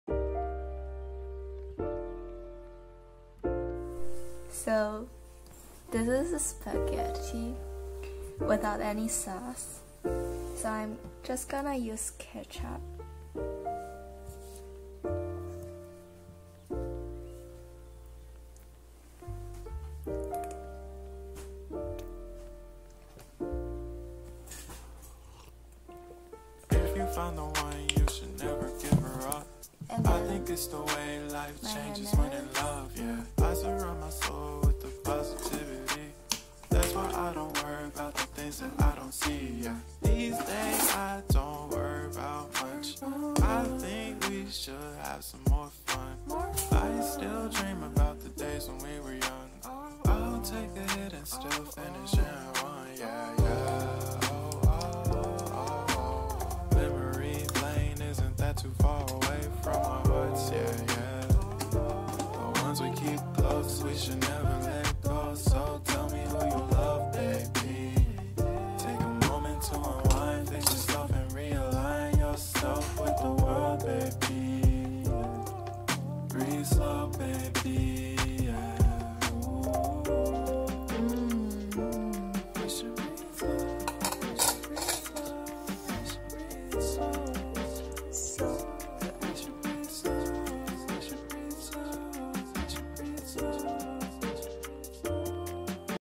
Putting ketchup and mayonnaise on spaghetti 🍝
Welcome to Olivia doing dumb stuff😑
Top quality spaghetti 🍝